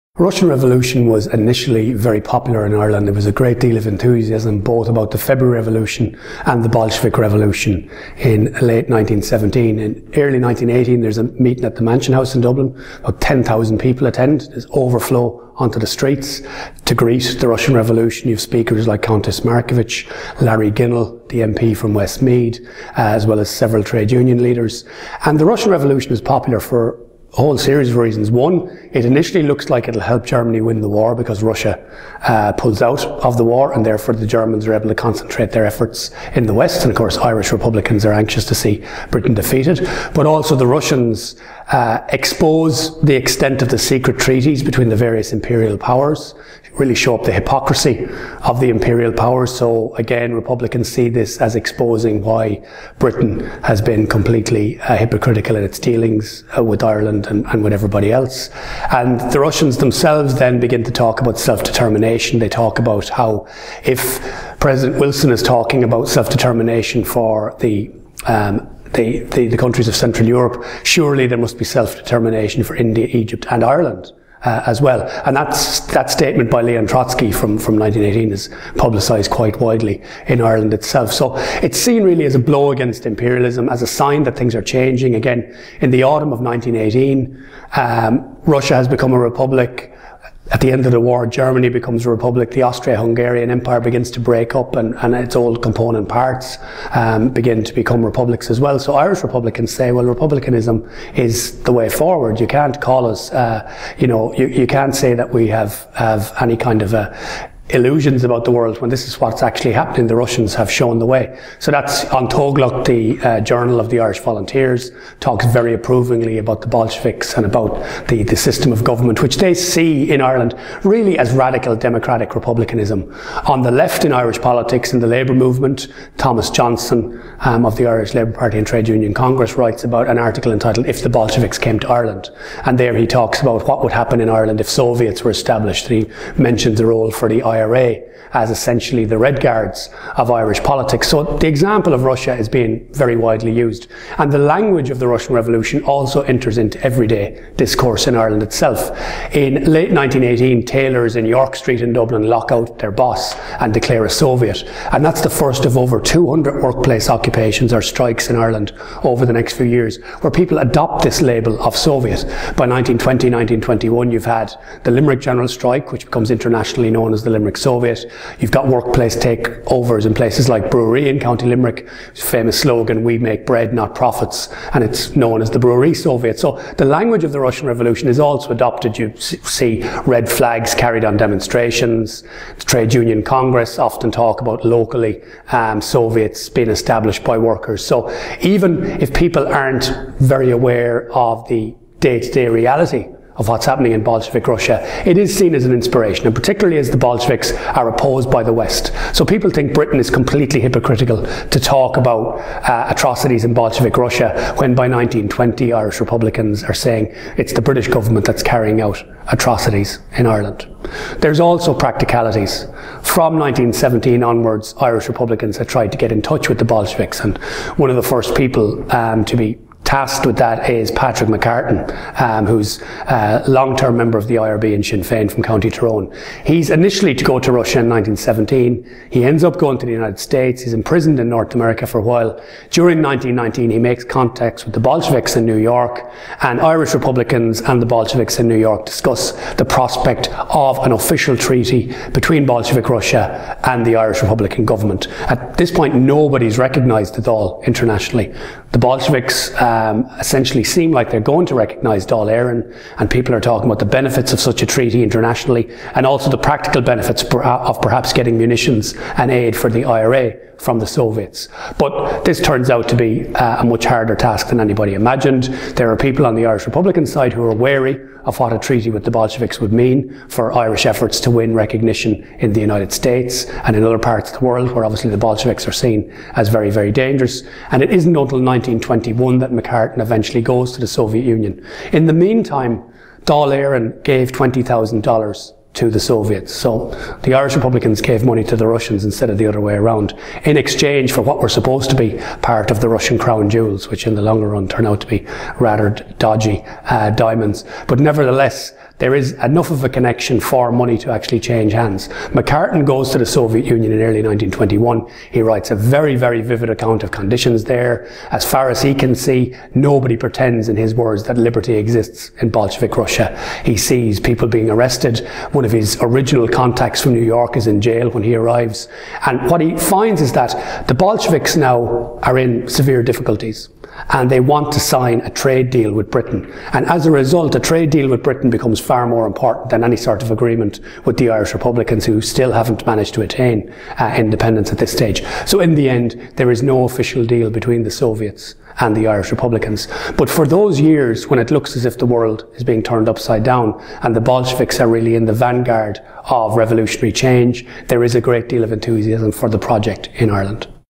Brian Hanley - Bolshevism in Ireland
Dr Brian Hanley, of the Global Irish Revolution project, describes the impact of the Russian Revolution in Ireland and on the Irish revolution